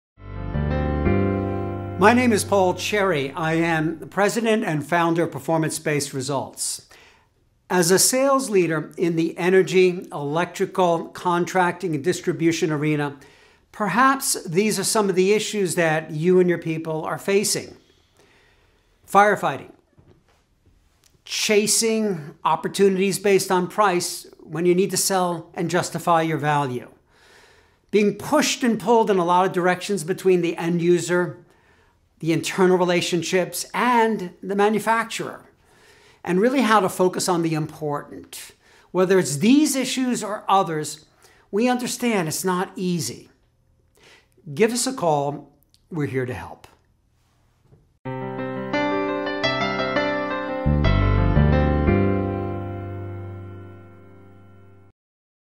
Energy Sales Training at PBR
Sales Challenges That Energy Companies Face
Sales representatives in the energy and power industry face a number of pain-points and are challenged by an extremely competitive marketplace, expanding technologies, and solutions dealing with climate change. Field‑based reps must establish and maintain a high level of customer satisfaction, while effectively managing prospects by developing sound marketing plans. Sales teams need to present solutions‑based models relying on ideas that supply value to the customer’s needs and pricing concerns.

That’s why Paul Cherry and Performance Based Results have developed customized electrical distribution, energy and power sales training programs that provide sales teams the skills to sell through the unique challenges of the industry — including how to help their customers embrace change, minimize risks, and be more profitable. Paul’s proven methods deliver expert industry sales training and coaching that guarantee results!